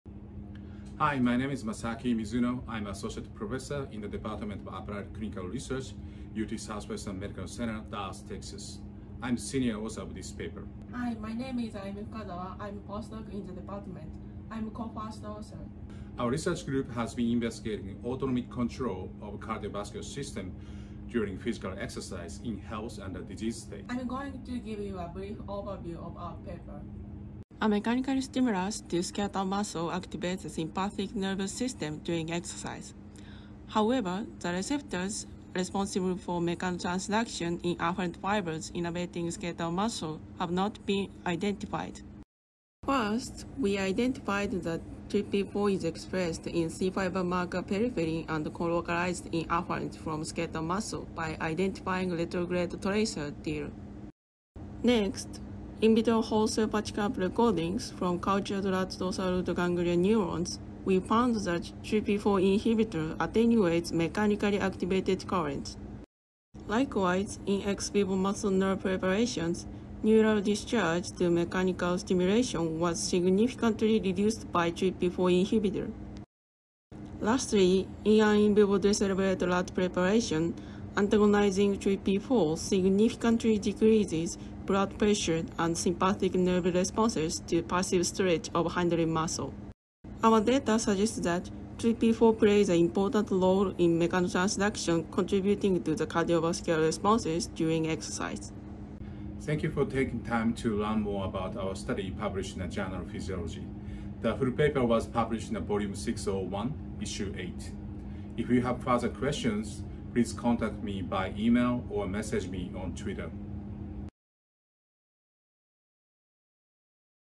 PHYSIOLOGY SHORTS: Antagonism of TRPV4 channels reduces mechanotransduction in rat skeletal muscle
In this Physiology Shorts video Masaki Mizuno and Ayumi Fukazawa (University of Texas) talks about their recent study 'Antagonism of TRPV4 channels partially reduces mechanotransduction in rat skeletal muscle afferents'.

Read more in The Journal of Physiology:
'Antagonism of TRPV4 channels partially reduces mechanotransduction in rat skeletal muscle afferents.'
Masaki Mizuno and Ayumi Fukazawa et al.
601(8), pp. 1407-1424

Transcript:
MM: Hi, my name is Masaki Mizuno. I am an Associate Professor in the Department of Applied Clinical Research at UT Southwestern Medical Center, Dallas, Texas. I am the senior author of this paper.

AF: Hi, my name is Ayumi Fukazawa, and I am postdoc in the department. I am co-first author.

MM: Our research group has been investigating autonomic control of the cardiovascular system during physical exercise in health and disease states.

AF: I am going to give you a brief overview of our paper.

Slide starts here

AF: A mechanical stimulus to skeletal muscle activates the sympathetic nervous system during exercise. However, the receptors responsible for mechanotransduction in afferent fibers innervating skeletal muscle have not been identified.

AF: First, we identified that TRPV4 is expressed in C-fiber marker peripherin and co-localized in afferent from skeletal muscle by identifying retrograde tracer, Dil.

AF: Next, in vitro whole-cell patch clamp recordings from cultured rat dorsal root ganglia neurons, we found that TRPV4 inhibitor attenuates mechanically activated currents.

AF: Likewise, in ex vivo muscle -nerve preparation, neural discharge to mechanical stimulation was significantly reduced by TRPV4 inhibitor.

AF: Lastly, in an in vivo decerebrate rat preparation, antagonizing TRPV4 significantly decreases blood pressure and sympathetic nerve responses to passive stretch of hindlimb muscle.

AF: Our data suggests that TRPV4 plays an important role in mechanotransduction contributing to the cardiovascular responses during exercise.

Slides ends here

MM: Thank you for taking time to learn more about our study published in The Journal of Physiology. The full paper was published in Volume 601, Issue 8.